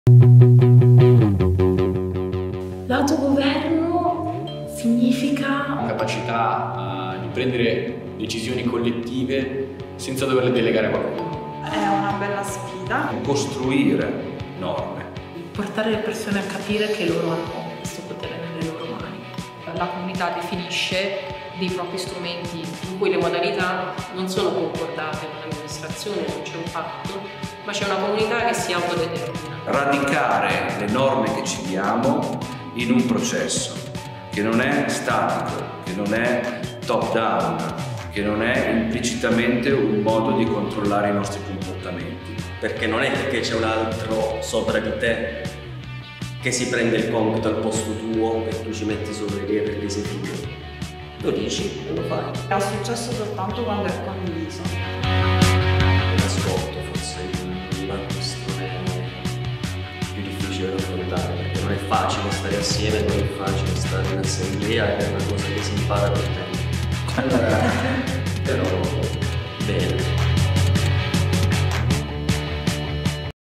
Glossario di comunità: "auto-governo"
Questo video è parte degli output del gruppo X nel laboratorio di ricerca e co-progettazione XYZSVMR a cura de La Scuola Open Source, in collaborazione con il laboratorio urbano Ex Fadda, a San Vito dei Normanni, nell'ambito del processo di rigenerazione urbana partecipata "Santu Vitu Mia Reloaded"